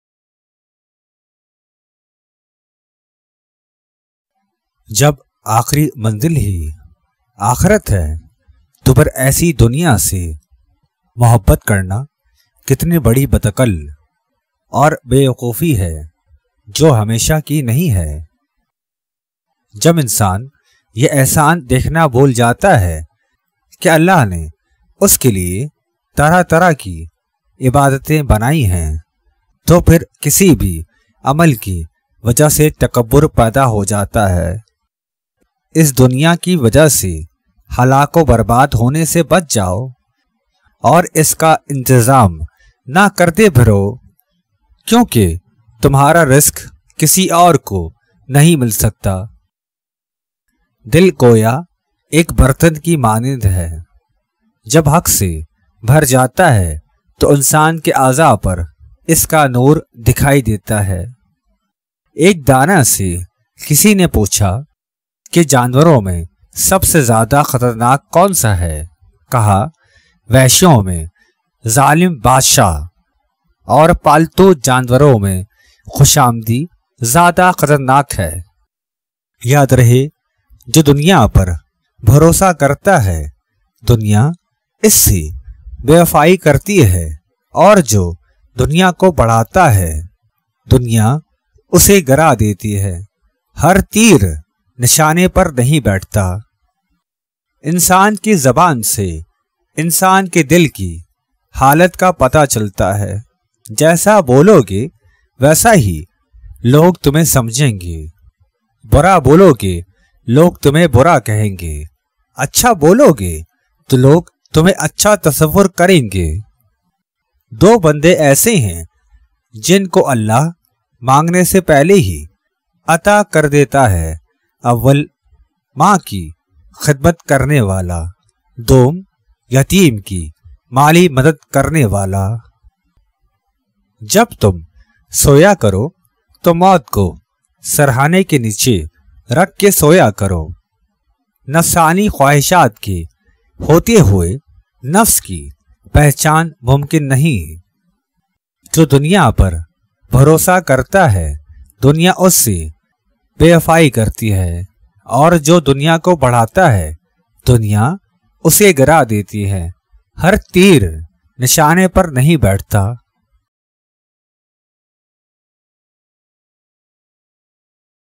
Insaan Ki Zabaan Sey Dil Ki Pata Chal Jata Hai | Sufism
Har Kaam Mein Allah Ka Shukar Karu Aur Musalsal Mehnat Karte Rahu Kamyabi Ki Nishani Hai.